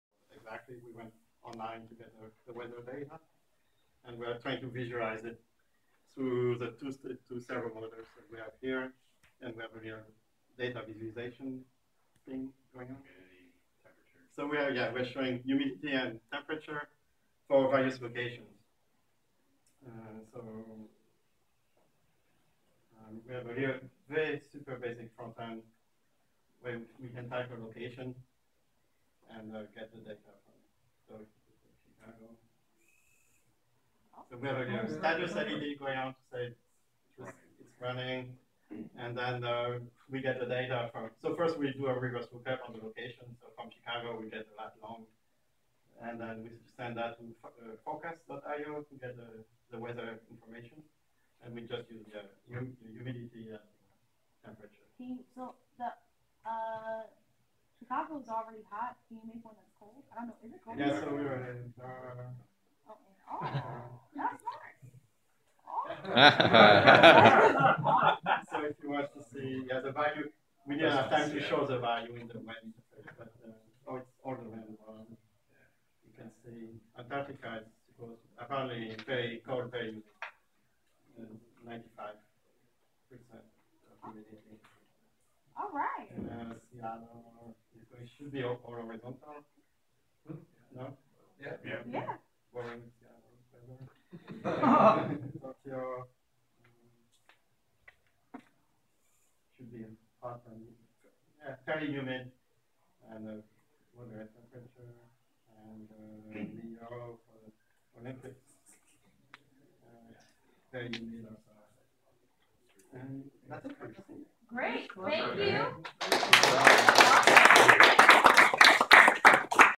NodeBots Day Chicago - Temp and Humidity Visualizer
Winner of the Weather Demo at NodeBots Day event at 1871 in Chicago on July 30, 2016. Powered by Node.js, Azure, Particle Photon, HiTec servos and art supplies.